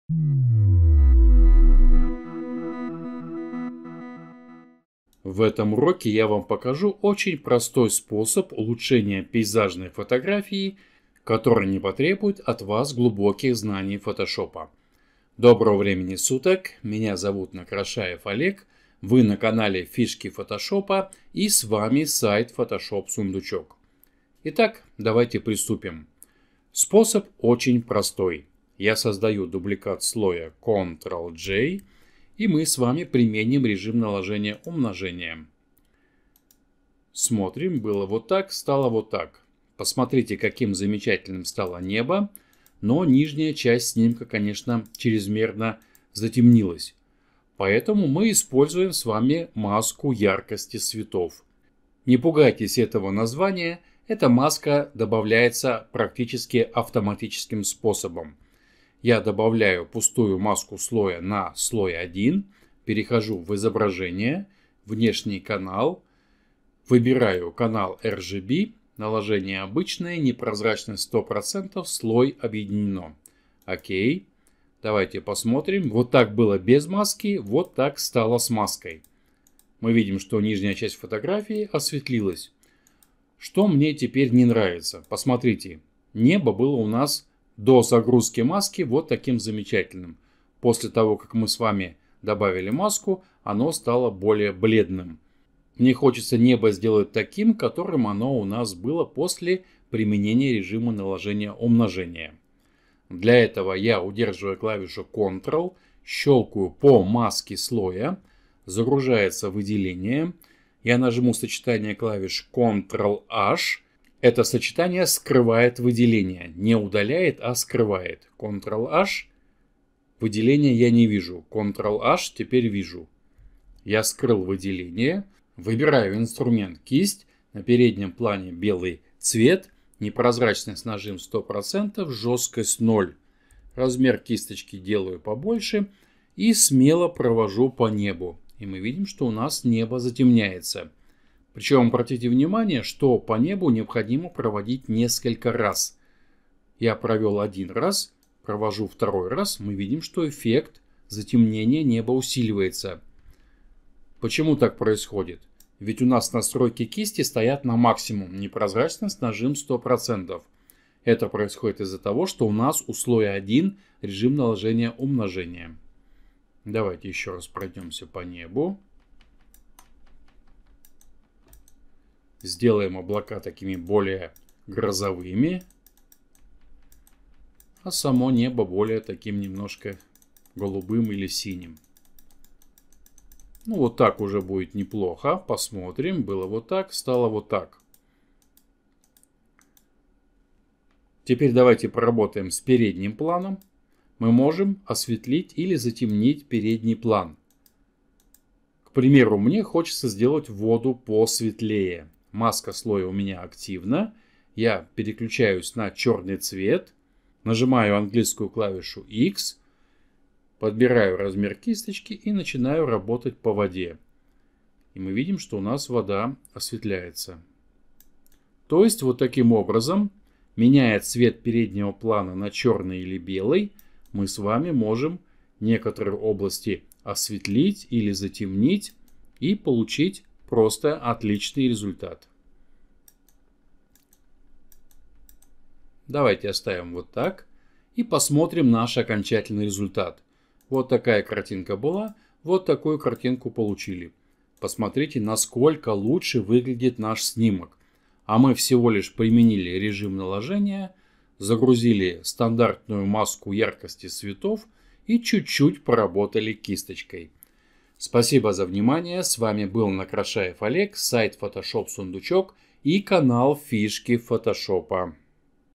Простой способ обработки пейзажного снимка
В этом уроке я Вам покажу очень простой способ улучшения пейзажной фотографии, который не потребует от Вас глубоких знаний фотошопа.

Автор канала - Накрошаев Олег